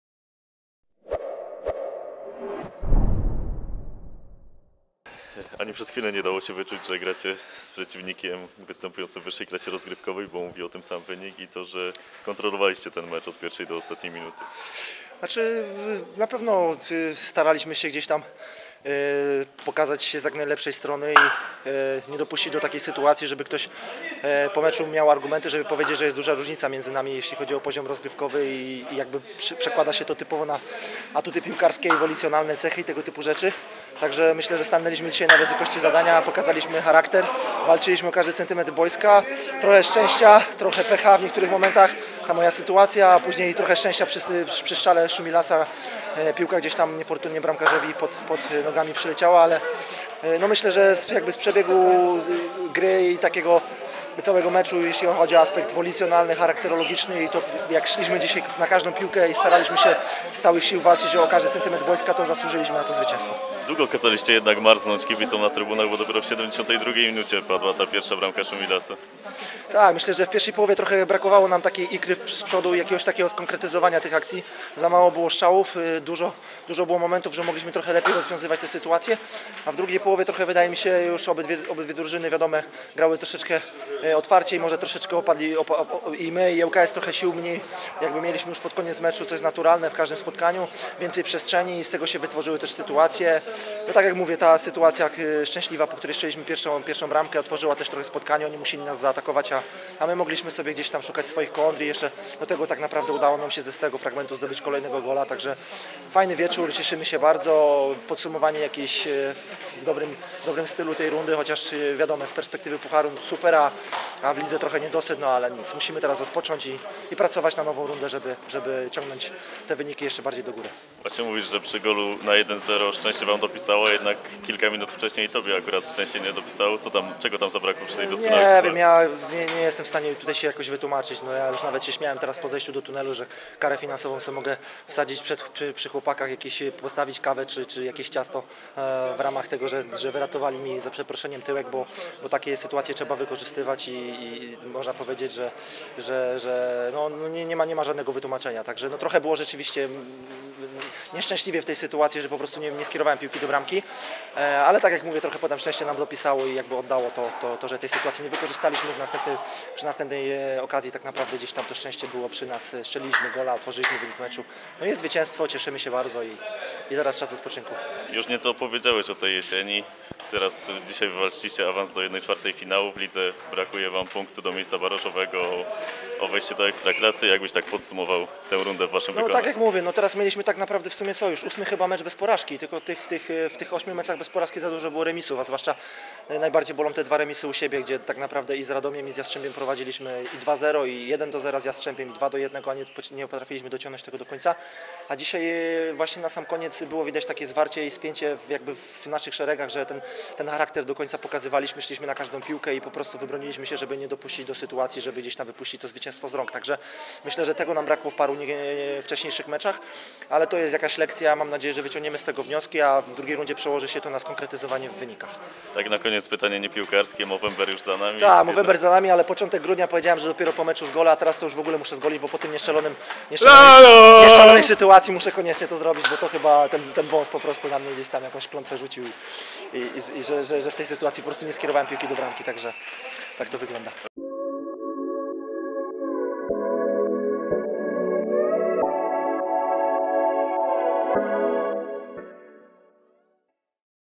GKS Tychy - ŁKS 2:0. Sebastian Steblecki: Zwrócę się o karę finansową dla siebie za tę sytuację
1/8 finału Pucharu Polski. Pierwszoligowy GKS Tychy pokonał Ekstraklasowicza, Łódzki Klub Sportowy, i zameldował się w ćwierćfinale rozgrywek. Posłuchajcie, co po spotkaniu miał do powiedzenia pomocnik gospodarzy, Sebastian Steblecki.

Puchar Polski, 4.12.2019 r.
GKS Tychy - ŁKS 2:0 (0:0)
1:0 Szumilas 72'
2:0 Biernat 80'